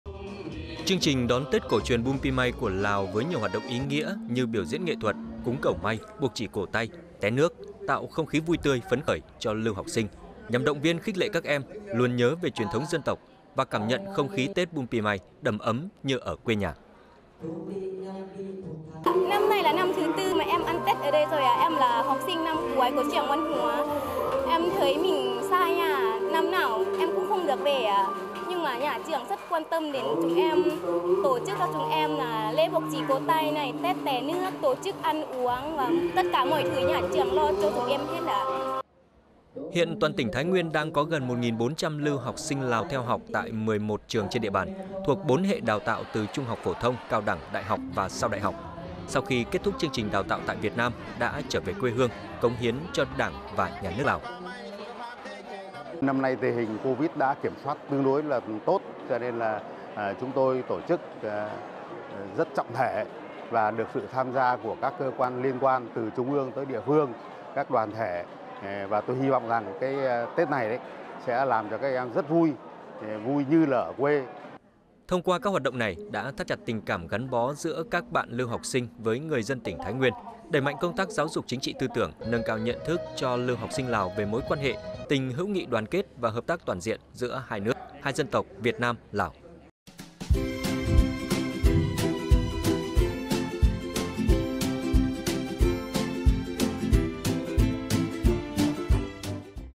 Tổ chức đón Tết Bunpimay cho lưu học sinh Lào | Bchannel
Nếu như cộng đồng dân tộc Khmer có Tết Chôl Chnăm Thmây, thì ở Lào, người dân xứ Vạn Tượng sẽ đón Tết Bunpimay. Và để chung vui với sinh viên Lào đang học tập tại Việt Nam, mới đây tại tỉnh Thái Nguyên, 1 chương trình ý nghĩa đã được tổ chức.